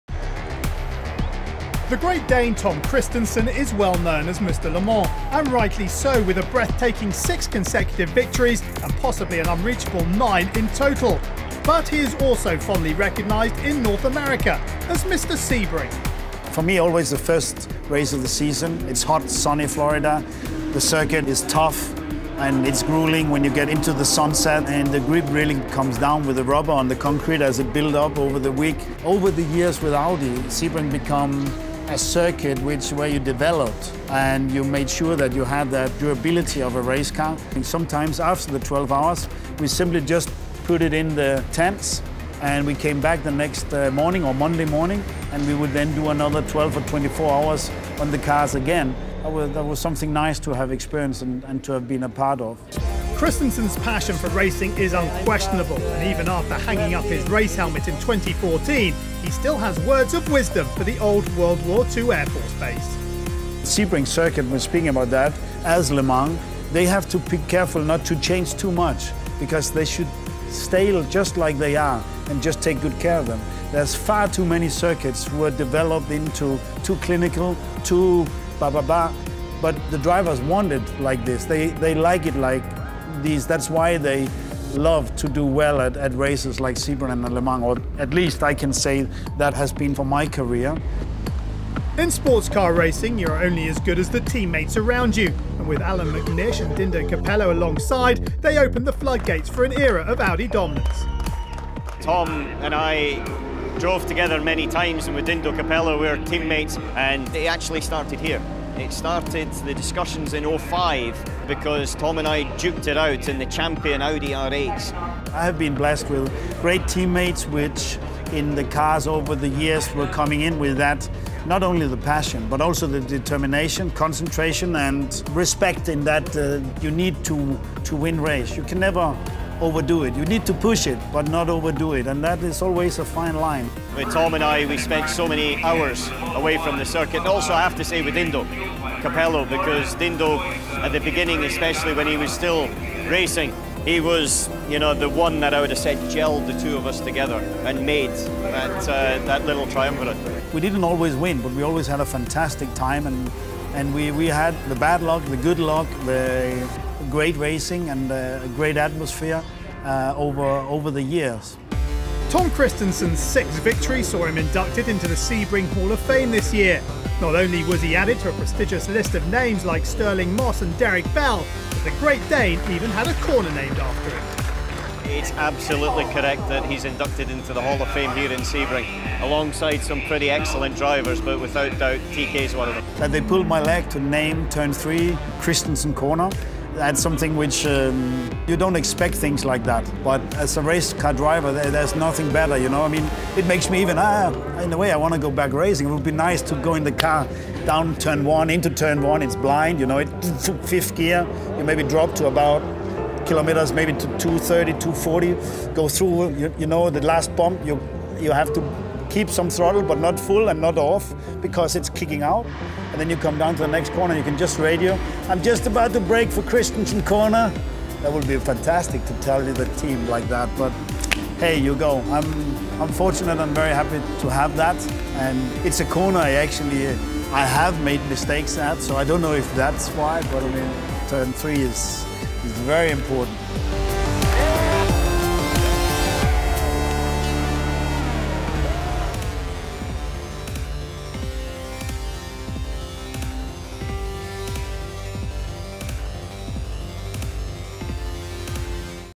A Great Dane – Tom Kristensen On Winning At Le Mans & Sebring | M1TG
Having been dubbed ‘Mr. Le Mans’ in view of an incredible 9 victories at the 24 Hours of Le Mans, Tom Kristensen also holds the record for the most outright race wins at the 12 Hours of Sebring (6). Mobil 1 The Grid caught up with the Dane, as well as his teammate Allan McNish, to talk endurance racing, Sebring and the infamous ‘Kristensen Corner’ named after him at Sebring Raceway.